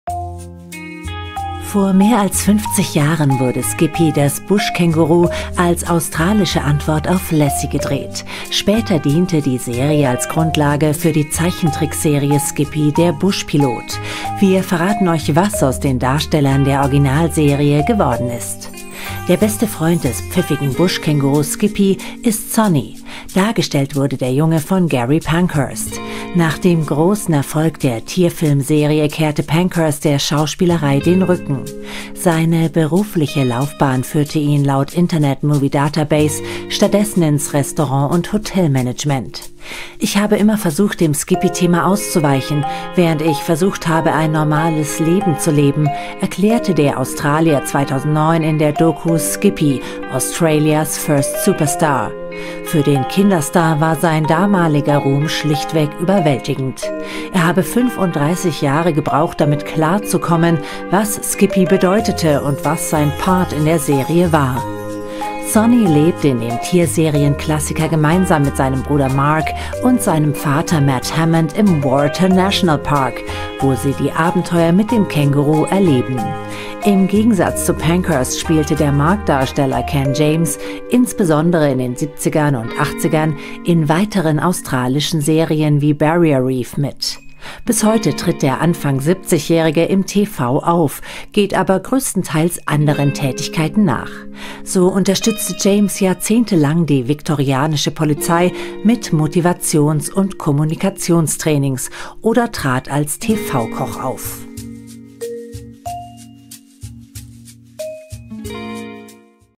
„Skippy, das Buschkänguruh“: Das machen die Stars heute – Teil 1 • PROMIPOOL
Vor mehr als 50 Jahren wurde „Skippy, das Buschkänguruh“ als australische Antwort auf „Lassie“ gedreht. Später diente die Serie als Grundlage für die Zeichentrickserie „Skippy, der Buschpilot“. Wir verraten euch, was aus den Darstellern der Originalserie geworden ist.